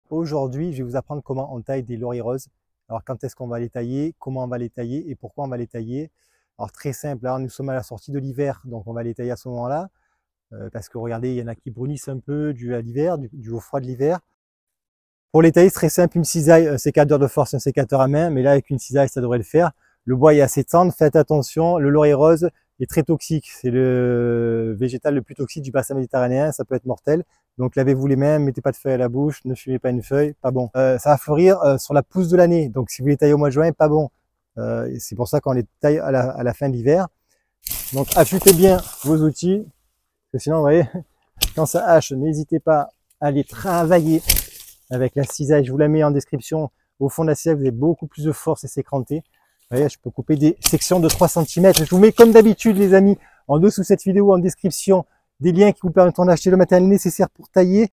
Quand tailler un Laurier Rose !
C’est une question qui revient très souvent.

Comment, quand et pourquoi tailler ses Lauriers Roses très simple regarde ce petit short en quelques secondes je réponds à ces trois questions.

À bientôt pour d’autres conseil,

Anthony

Je te mets les liens d’outils de base à avoir pour tailler :

- Petit sécateur

- Gros sécateur